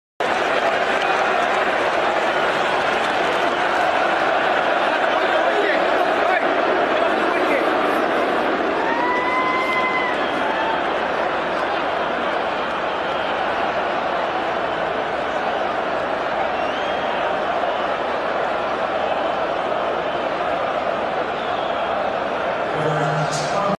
Cricket Sport ind vS eng, jarvo
new player India team
naya khiladi India k
out come jarvo